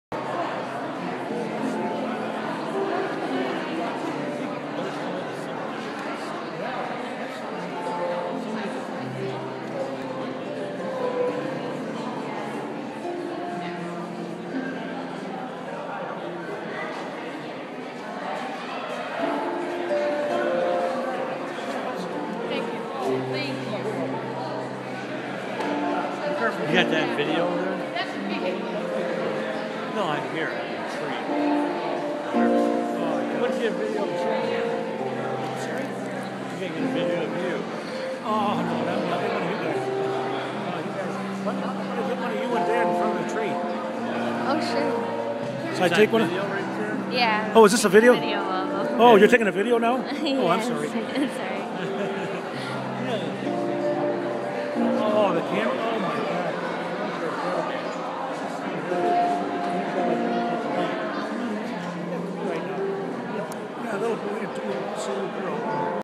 12-10-2011 Irish harp player
My moms Christmas memorial 3rd year a harp player plays some beautiful Gaelic music in Cadillac memorial gardens east moselum